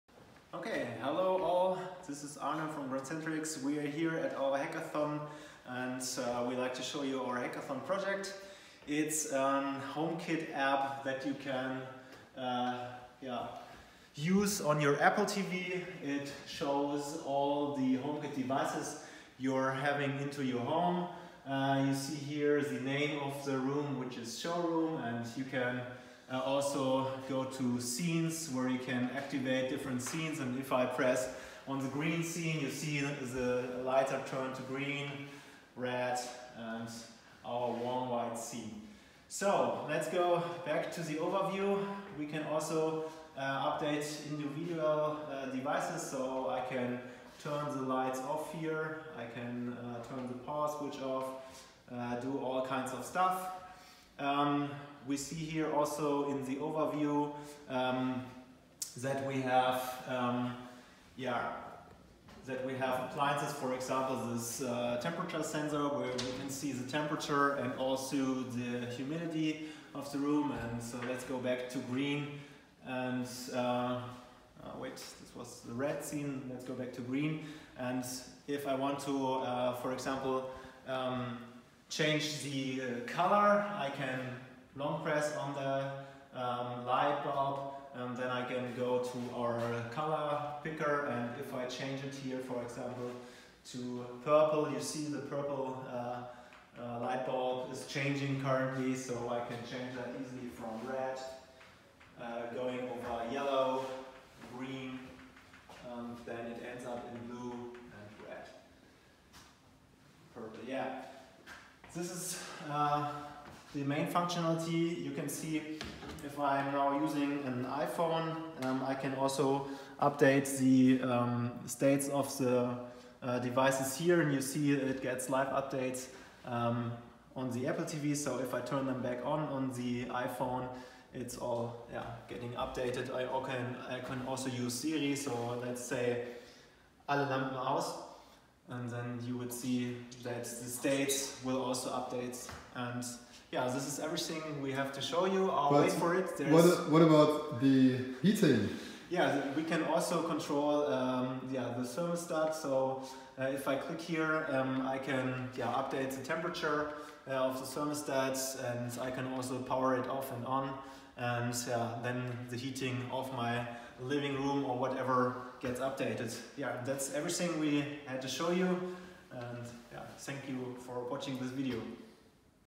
HomeKit on Apple TV "shipping" after just 8 hours hacking at grandcentrix
It's this time of the year again: The great grandcentrix IoT and Smart Product hackathon! Our very own Erid Bardhaj, Stefan Horst, Timo Josten and Arne Tempelhof are bringing HomeKit to Apple TV in just 8 hours of intensive hacking.

Do you want to bring HomeKit to your product portfolio? Our team is here to help!